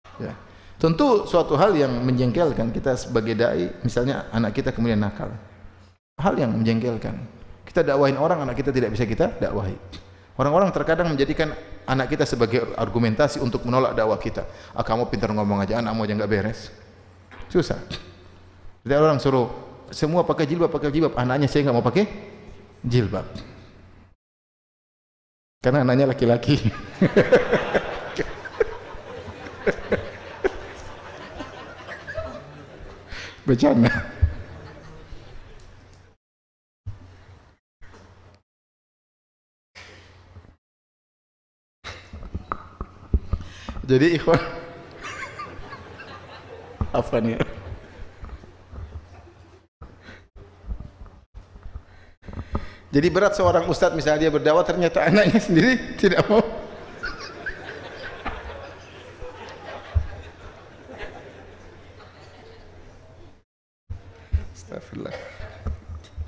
Anak Tidak Mau Pakai Jilbab - Ustadz Dr. Firanda Andirja, M.A
📝 Judul kajian: (TELADAN KESEHARIAN DARI KISAH PARA NABI ALAIHIMUSSALAM)
🎙️ Penceramah: Ustadz Dr. Firanda Andirja, Lc., M.A. حفظه الله تعالى